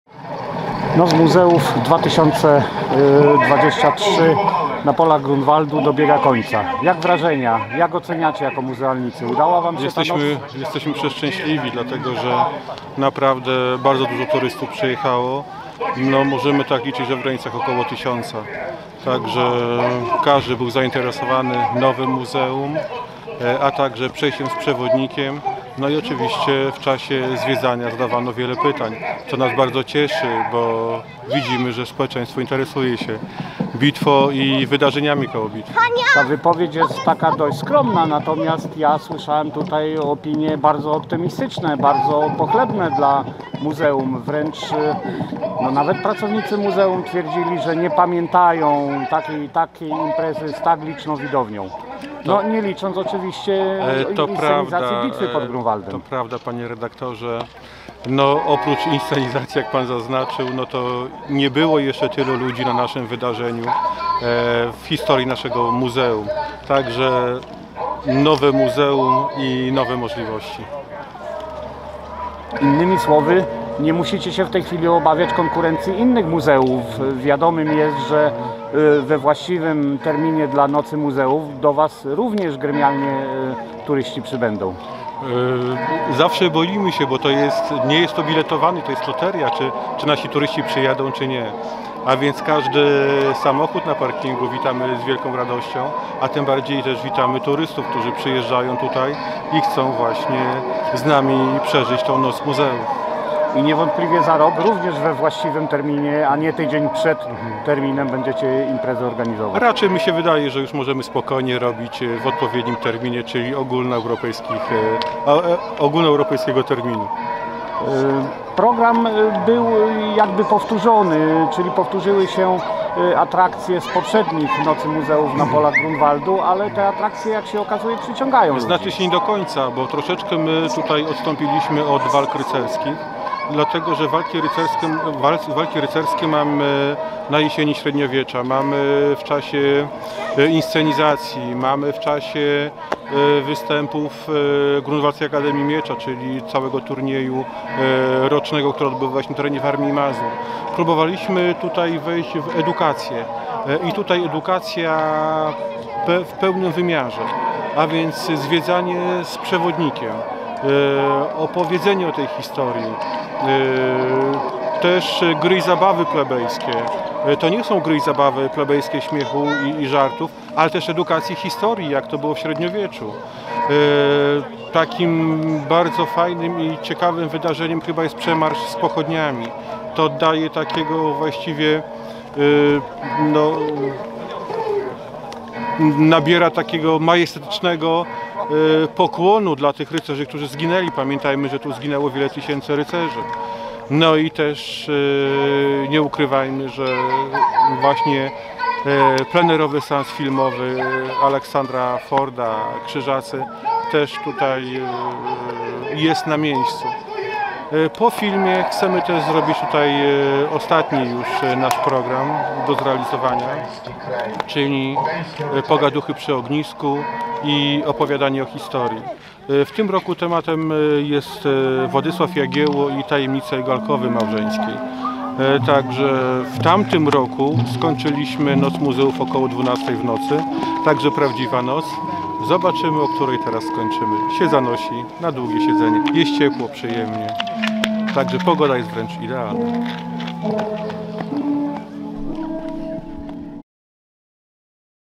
Jarosław Malecki o Nocy Muzeów 2023 w Muzeum Bitwy pod Grunwaldem
Wypowiedź Jarosława Maleckiego z Muzeum Bitwy pod Grunwaldem o przebiegu Europejskiej Nocy Muzeów | Muzeum Bitwy pod Grunwaldem | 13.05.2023